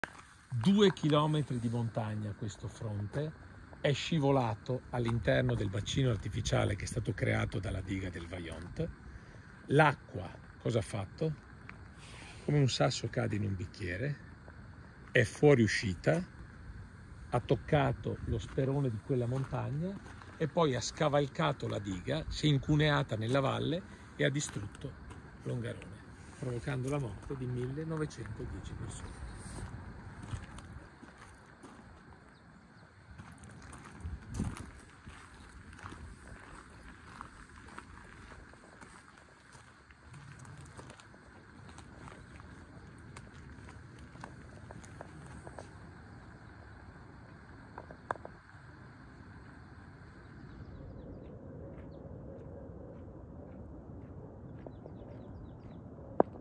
La diga del Vajont una tragedia da non dimenticare
La tragedia del Vajont sul luogo del disastro del 1963